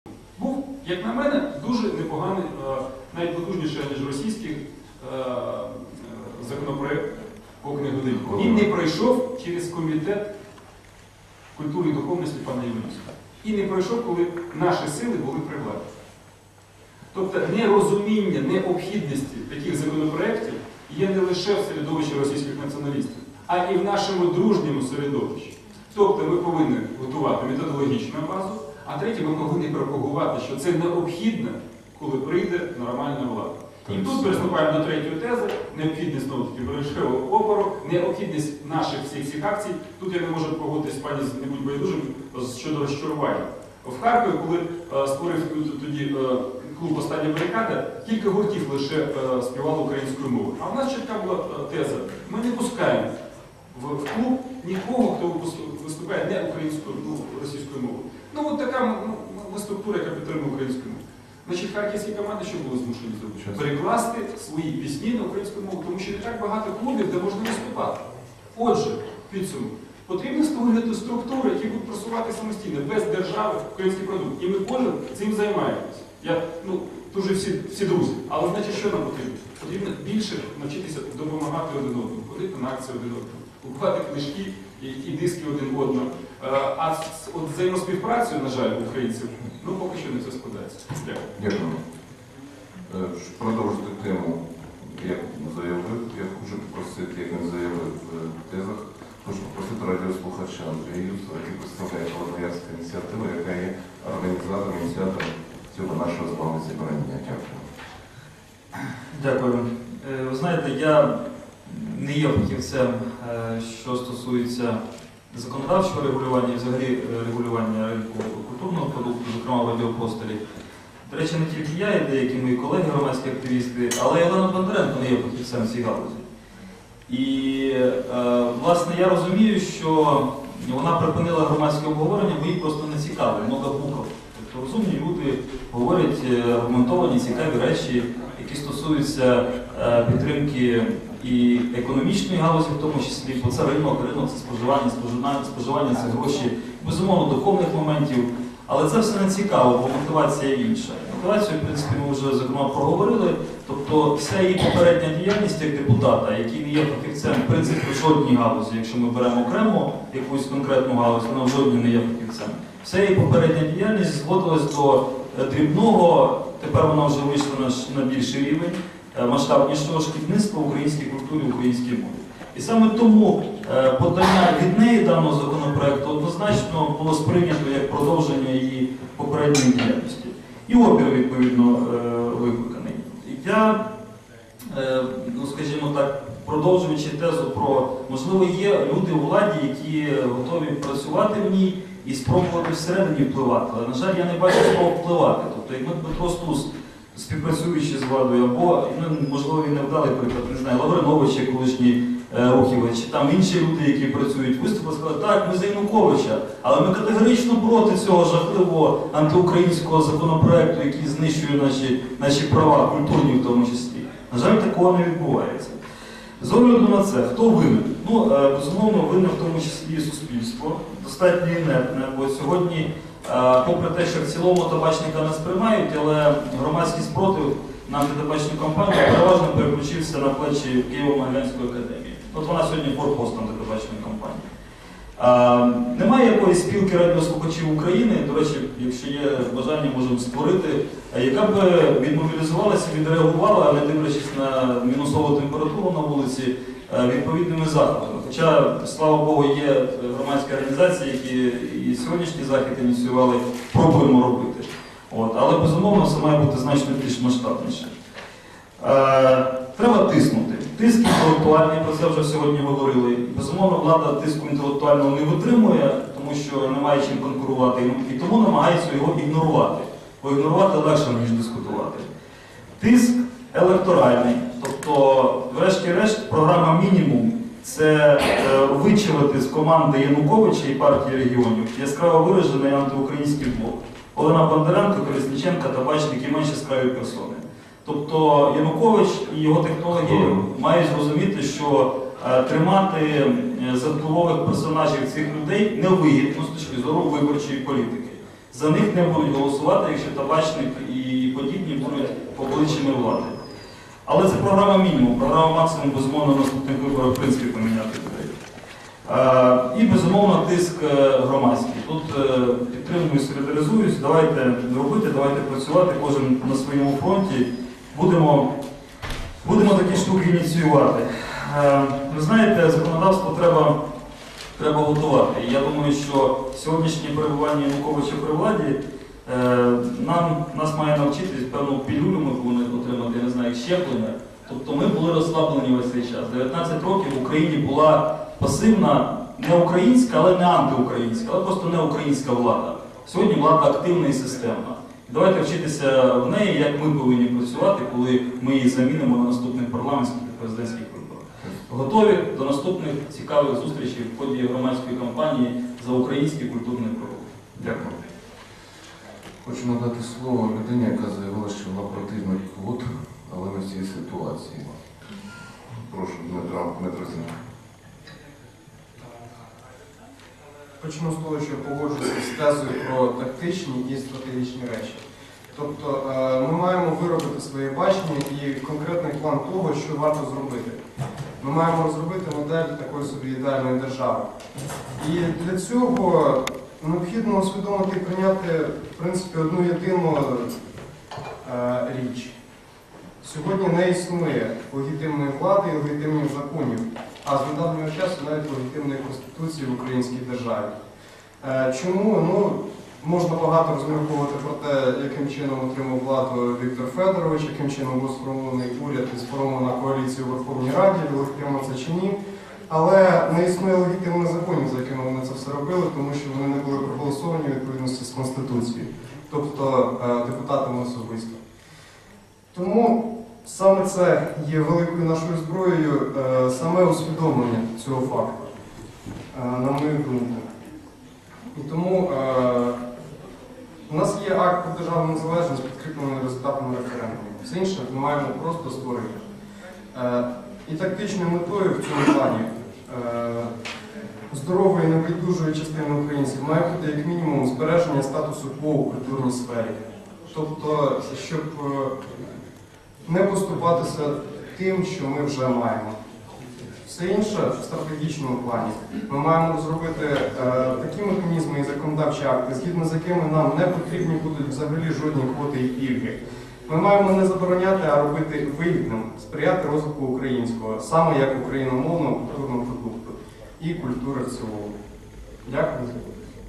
Доповіді (ч.5)
Олесь Доній - народний депутат України VI скликання, НУНС;
Андрій Юсов - "Холодноярська ініціатива";
Дмитро Сінченко - громадська ініціатива "Воля";